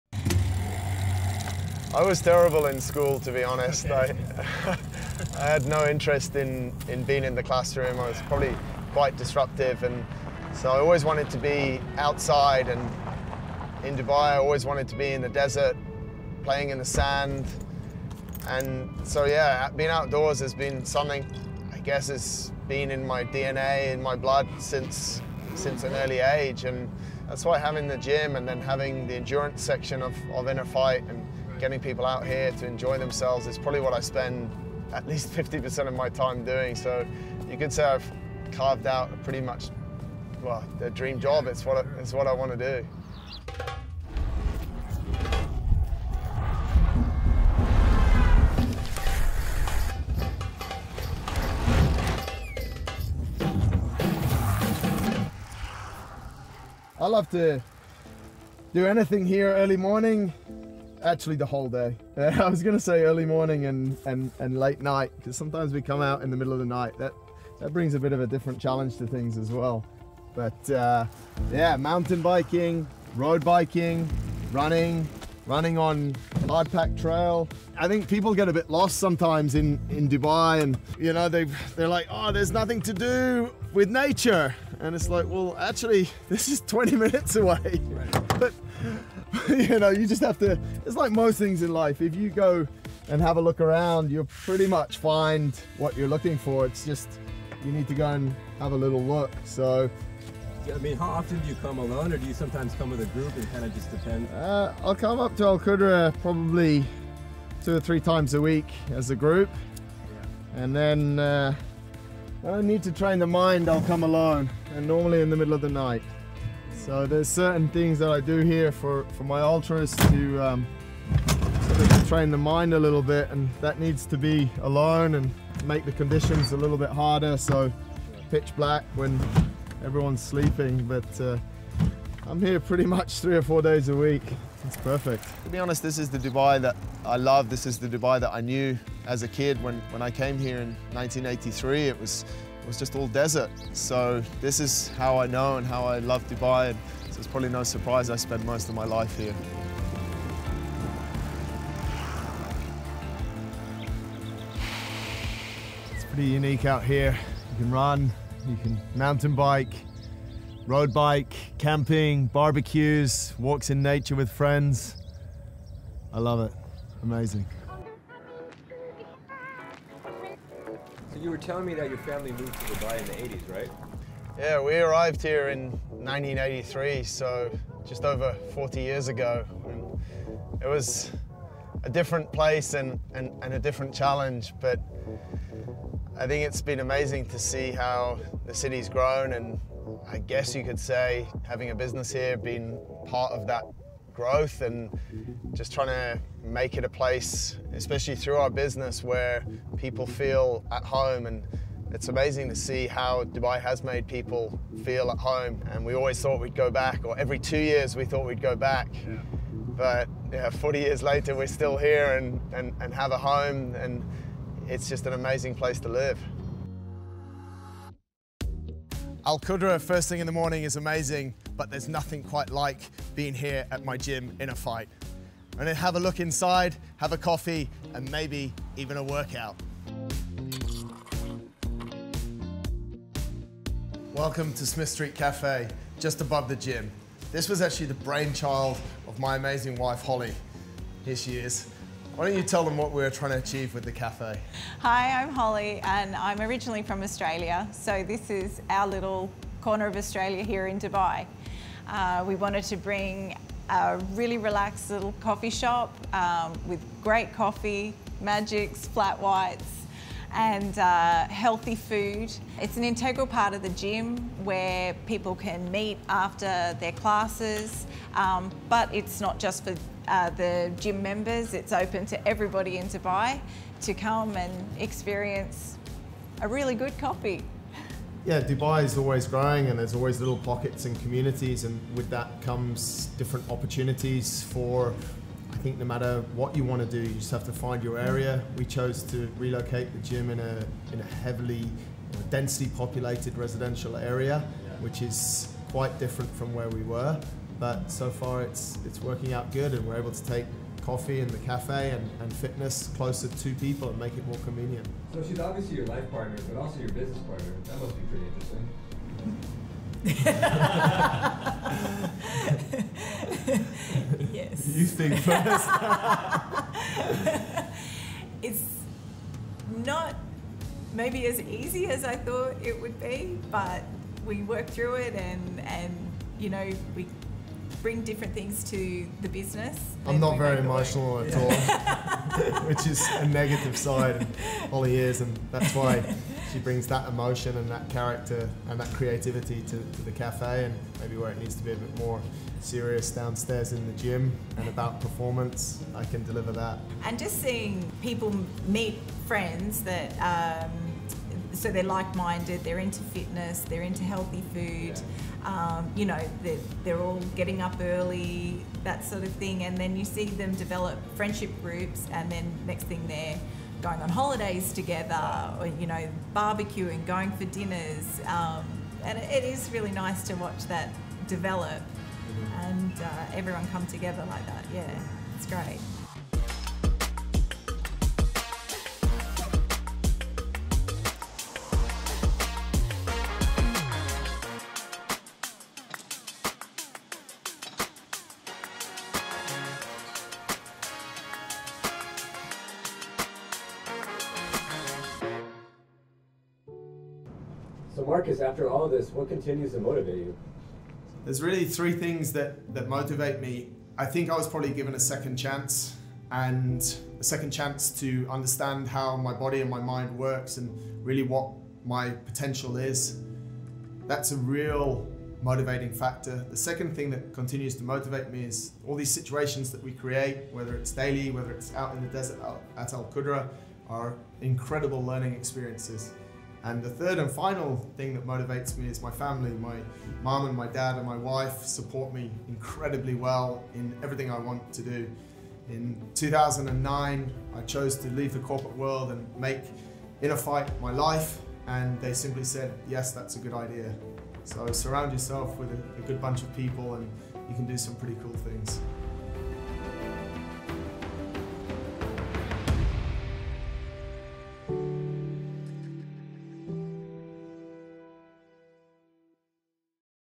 Cycling and Running in Dubai With Athlete & Entrepreneur Marcus Smith 🚴🏋🏽☕
From cycling at Al Qudra Desert to running and mountain biking, there are so many ways for fitness enthusiasts to enjoy sports in Dubai. Watch as InnerFight Gym’s Marcus Smith shares how the city strengthened his athletic passion, leading him to create a thriving business in Dubai Studio City.

0:00 Intro
0:14 Al Qudra Desert
3:37 InnerFight Gym | Smith St Paleo & Café
6:36 Marcus Motivation
7:33 Subscribe